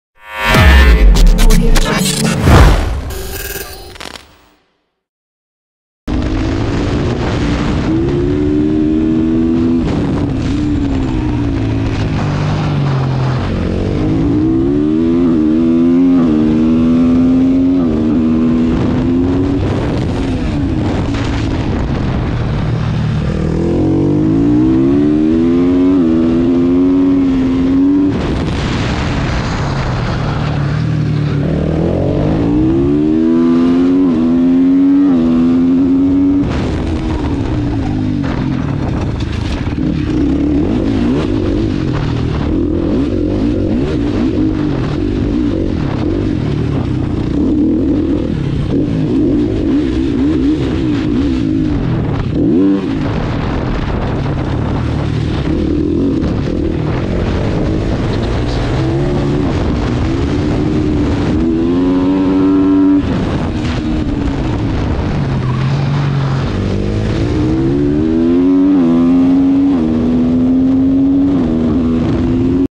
A fast lap with Swedish Supermoto Champion of 2021 Hampus Gustafson, Tuvängen Ring
We ride onboard with Hampus on his GasGas as he sets a decent laptime.